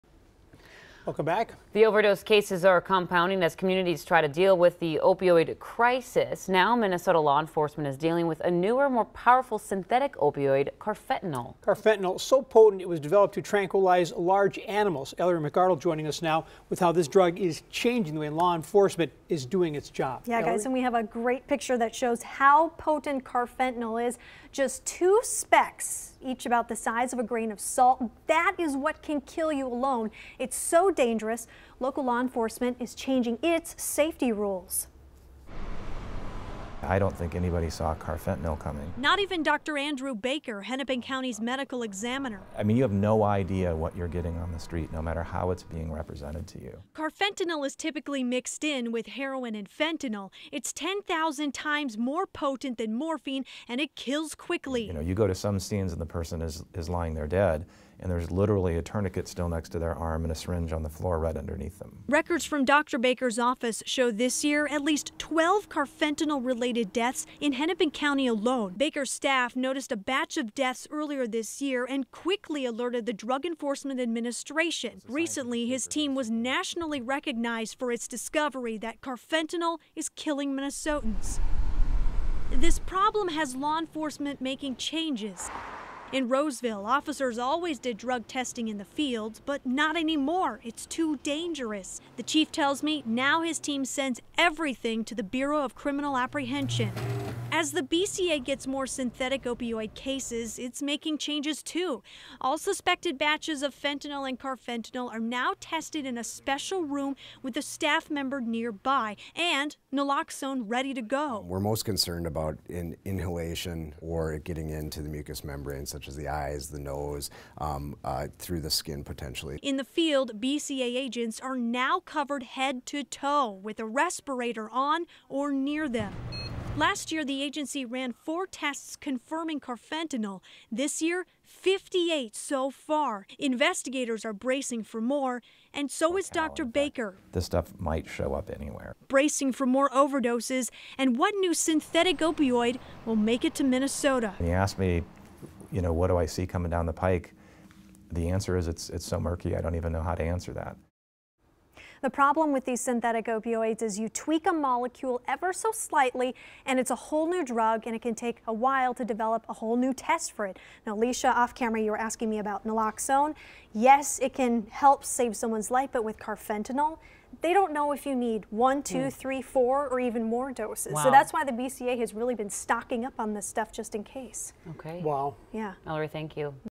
Carfentinal new challenge for MN law enforcement
MN authorities are facing a new foe in the drug battle, a large animal tranquilizer known as Carfentinal.